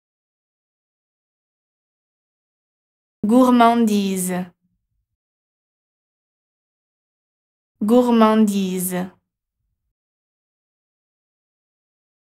How to pronounce GOURMANDISE in French
This video shows you how to pronounce GOURMANDISE in French. Speaker has an accent from Calais (Pas-de-Calais, Hauts-de-France). Collins Dictionary, the home of living language and pioneers of dictionary publishing